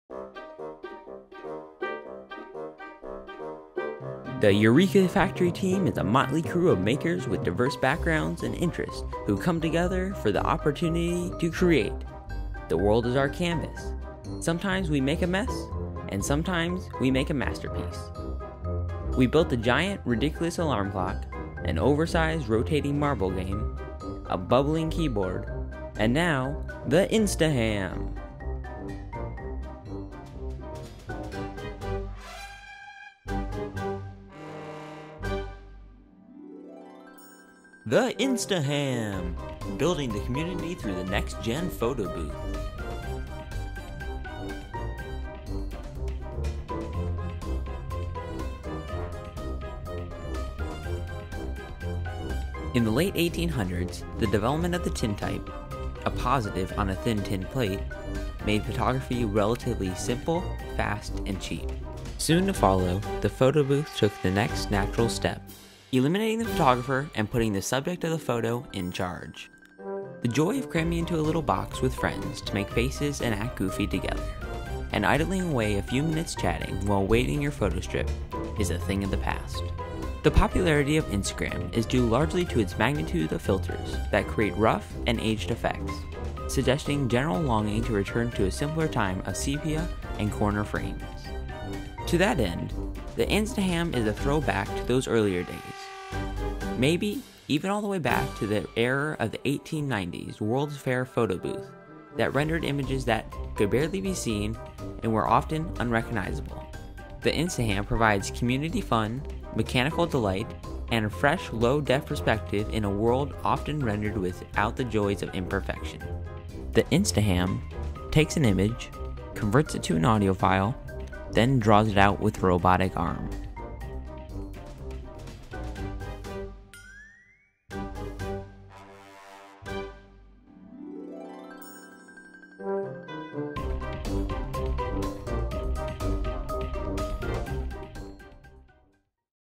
The InstaHam by Team Eureka Factory - Red Bull Creation 2014 Qualifier Entry
For Red Bull Creation 2014, we answered the challenge to makes the world a better place, a more fun and interesting place, by putting a new age spin on an old tyme tradition: the  photo booth, a social experience where the outcome was always a surprise and you had to wait for the magic to happen.  Our goal is not only achievable, but we already achieved it, building a prototype for our qualifying video we call the InstaHam.

We built the InstaHam from a TRS Drawbot from instructions on the Make Magazine blog with a small computer and a webcam to create a portable robotic photo booth that can be taken to various events as an outreach tool of both Eureka! Factory and our FIRST Tech Challenge robotics team.  By taking the InstaHam into the community we can engage people with technology in a fun and non-threatening way. The simple drawing mechanism helps to demystify robotics and computer aided manufacturing as well as demonstrating how accessible technology and making is to the general public. Finally, this project is intended to make people smile and spread a sense of wonder,  after all, you just got your portrait done by a robot!  How cool is that?

Shot Ryder
Edited Ryder